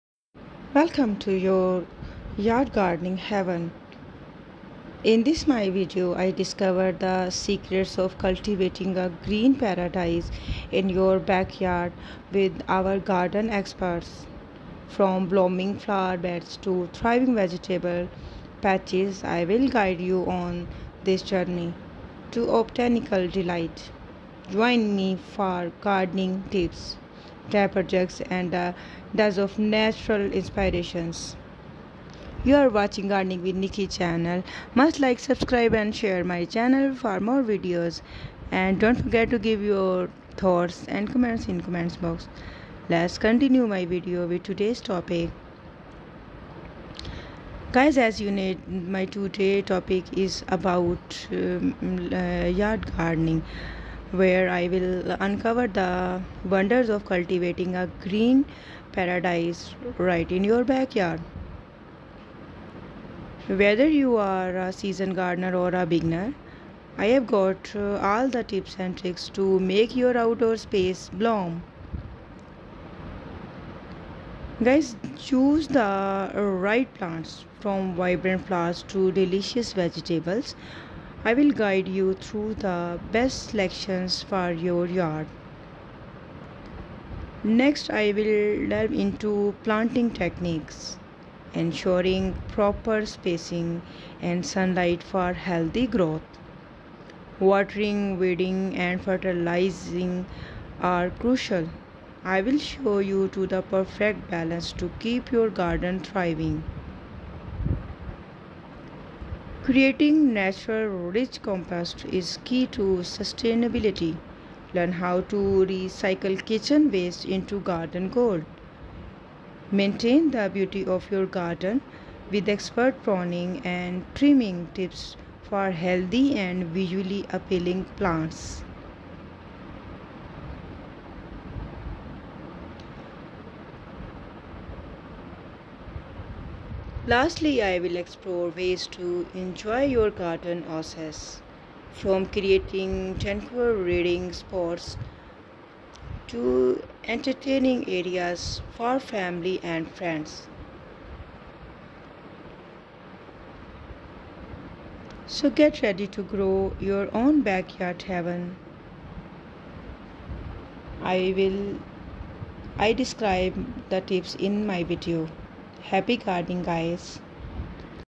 100 Yard Gardening Secrets|| Unveiled Yard Gardening Ideas for a Beautiful Oasis.
In my today's video i discover the secrets of cultivating a green paradise in your backyard with our gardening experts. From blooming flower beds to thriving vegetable patches, i'll guide you on this journey of botanical delight.